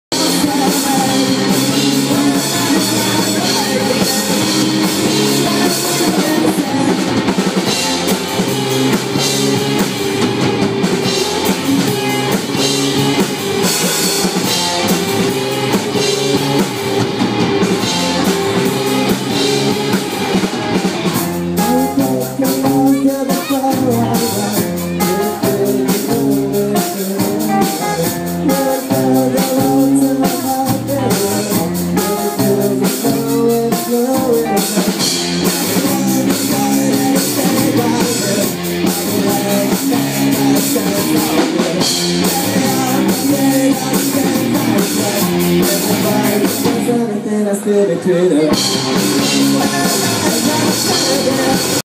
Fête de la musique à Saint-Manvieu-Bocage-La Voix Le Bocage
Le groupe cherbourgeois Acid river side.